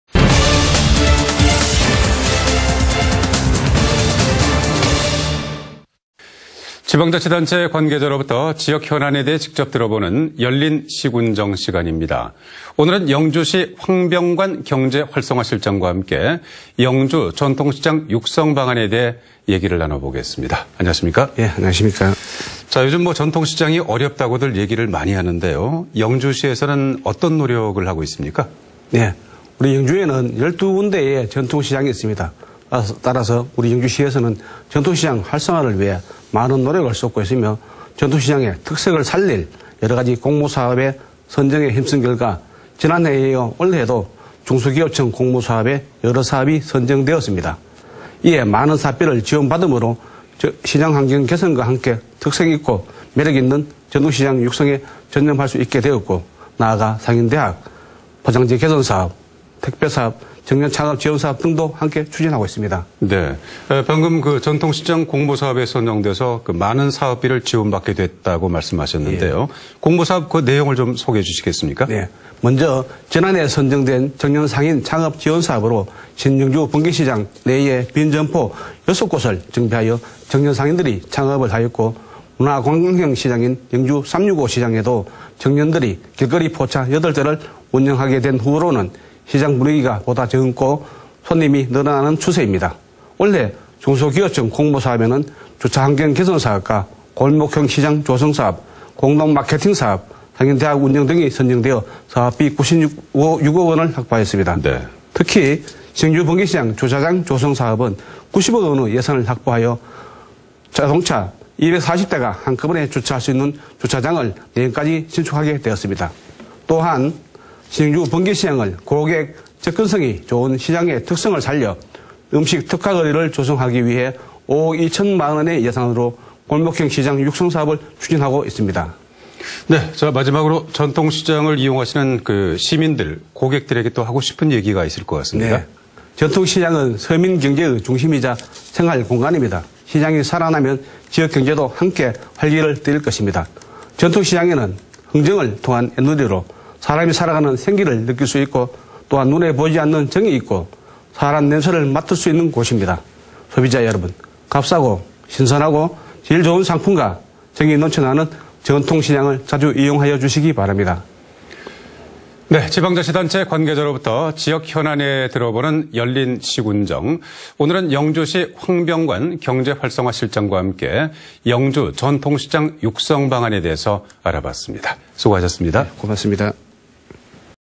[안동MBC뉴스]R]열린 시.군정 '영주시' 7.전통시장 활성화
2017/04/11 10:30:37    작성자 : 홍석준

◀ＡＮＣ▶

지방자치단체 관계자에게 지역 현안에 대해
직접 듣는 '열린 시군정' 시간입니다.

오늘은 영주시청 황병관 경제활성화실장과
영주 전통시장 육성방안에 대해
이야기 나눠보겠습니다.
◀ＥＮＤ▶

- 요즘 전통시장이 어렵다고 얘기를 많이 하는데, 영주시에서는 어떤 노력을 하고 있는지요?

= 우리 영주에는 12개소의 전통시장이 있습니다. 따라서 영주시에선 전통시장 활성화를 위해 많은 노력을 쏟고 있으며, 전통시장의 특색을 살릴 여러 공모사업 선정에 힘쓴 결과, 지난해에 이어 올해도 중소기업청 공모사업에 여러 사업이 선정되었습니다.

이에 많은 사업비를 지원 받음으로, 시장환경 개선과 함께 특색있고 매력있는 전통시장 육성에 전념할 수 있게 되었고, 나아가, 상인대학과 포장재 개선사업, 택배사업과 청년창업 지원사업 등도 함께 추진하고 있습니다.

- 방금, 전통시장 공모사업에 선정되어 많은 사업비를 지원 받게 되었다고 하셨는데 공모사업의 내용 좀 소개 해주시겠습니까?

= 먼저, 지난해 선정된 청년상인 창업지원 사업으로 신영주 번개시장 내에 빈 점포 6개소를 정비하여 청년 상인들이 창업을 하였고, 문화관광형 시장인 '영주 365 시장'에도 청년들이 길거리 포차 8대를 운영하게 된 후로는 시장 분위기가 보다 젊고, 손님이 늘어나는 추세입니다.

올 해, 중소기업청 공모사업으로는 주차환경개선사업, 골목형시장 조성사업, 공동마케팅사업, 상인대학 운영 등이 선정돼 사업비 96억원을 확보하였습니다.

특히, 신영주번개시장 주차장 조성 사업은 90억원의 예산이 확보되어 240대가 한꺼번에 주차할 수 있는 주차타워를 내년까지 신축할 수 있게 되었습니다.

또한, 신영주 번개시장을, 고객 접근성이 좋은 시장의 특성을 살려서 음식 특화거리를 조성하기 위해 5억 2천만원의 예산으로 '골목형시장 육성사업'을 추진하고 있습니다.

- 전통시장을 이용하시는 시민들, 고객들에게 당부의 말씀 부탁드립니다.

= 전통시장은 서민경제의 중심이자 생활공간입니다. 시장이 살아나면 지역경제도 활기를 띌 것입니다. 전통시장에는 흥정을 통한 에누리로 사람이 살아가는 생기를 느낄 수 있으며, 눈에 보이지 않는 정(情)이 있고 사람 냄새를 맡을 수 있는 곳입니다.

소비자 여러분께 값싸고 신선하고 질 좋은 상품과 정이 넘쳐나는 전통시장을 자주 이용해 주시길 부탁드립니다.

- 오늘은 영주시 황병관 경제활성화실장과 함께 영주 전통시장 육성방안에 대해 알아봤습니다.